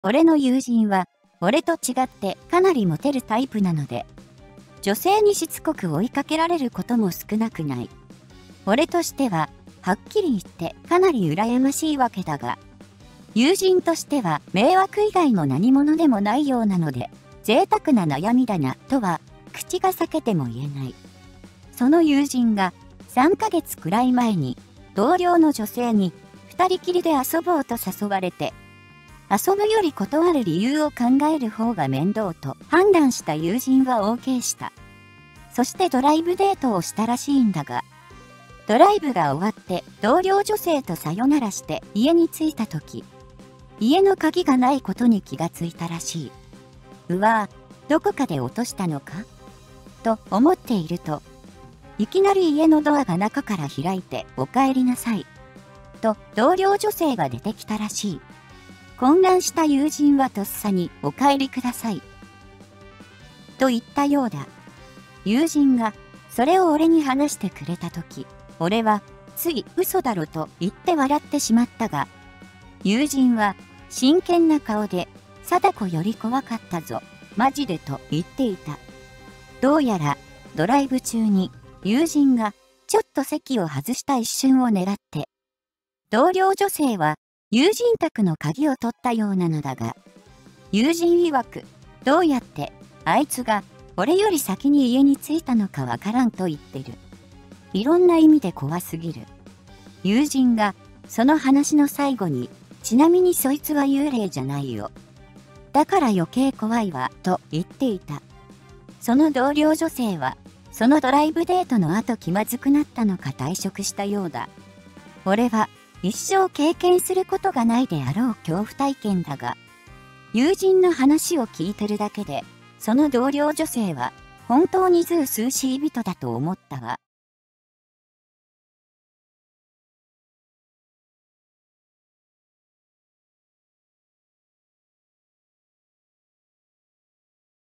【修羅場】 友人が同僚の女性に「二人きりで遊ぼう」と誘われて、「女性とドライブしたんだけど…」俺「嘘だろｗ」「幽霊じゃないから余計怖い」【痛快・スカッとジャパン】
※動画音楽
　LAMP BGM　様